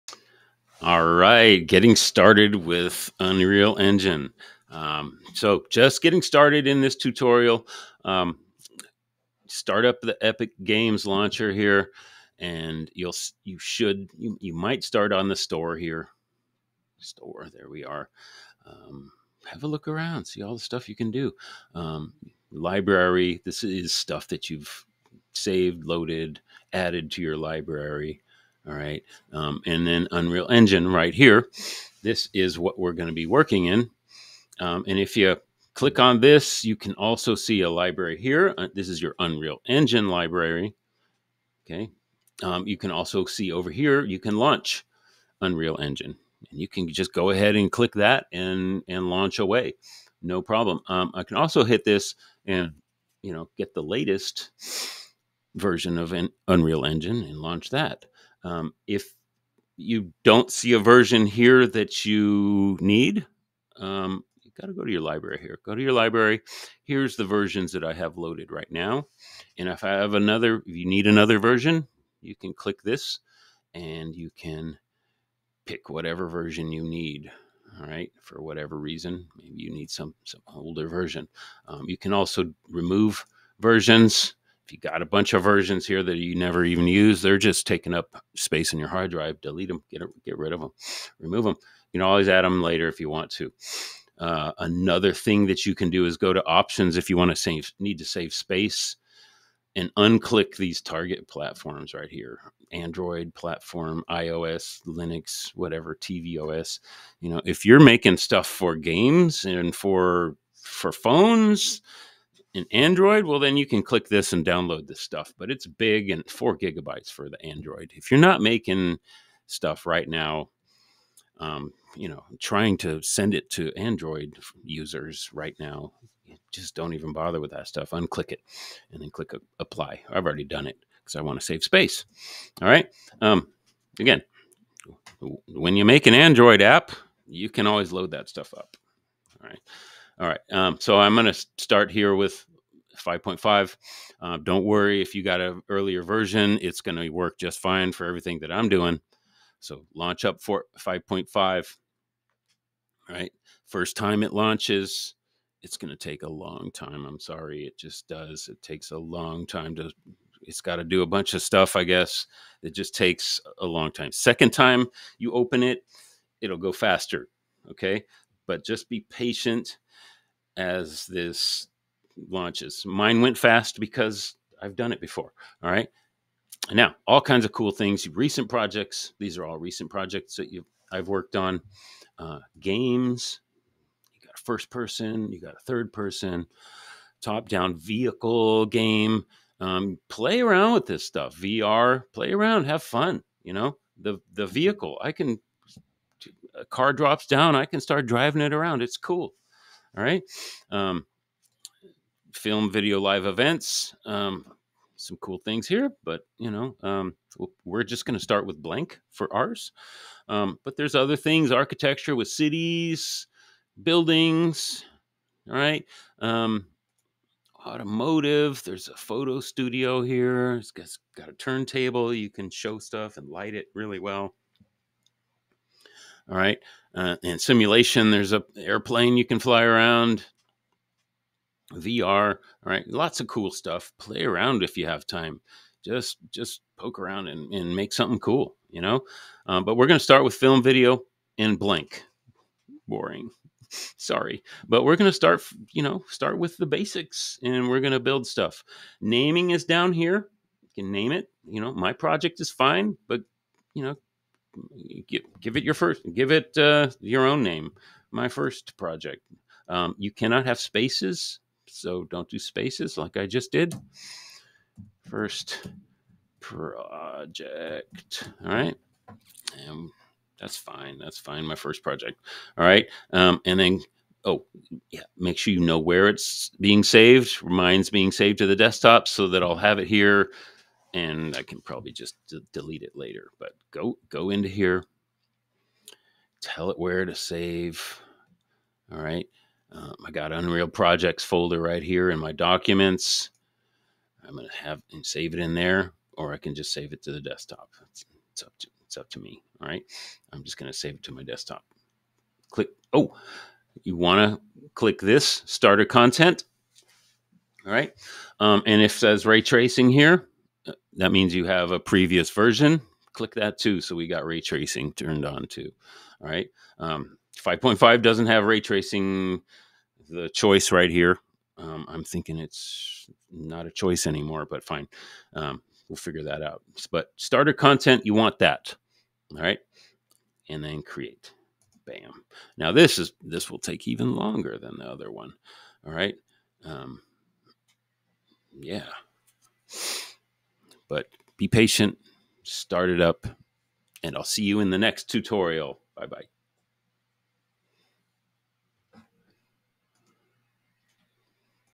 Beginner's Guide to Unreal Engine 5.5: Getting Started Tutorial
In this tutorial video, I guide you through the process of starting with Unreal Engine. I show you how to navigate the Epic Games launcher, select and launch Unreal Engine, manage versions, and optimize space by deselecting target platforms. I demonstrate creating a new project with naming and saving options, as well as enabling starter content. I stress the importance of patience during the initial launch and encourage exploring the various project types and features available in the software. Join me as I walk you through the steps to kickstart your journey with Unreal Engine!